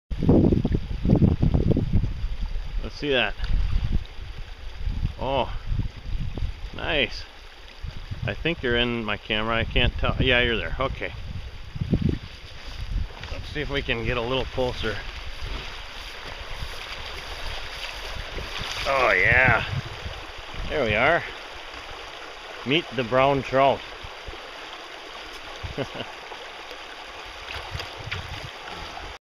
Ich releases a brown trout
Fly fishing the spring creeks is a blast...we never keep any. Strictly catch and release.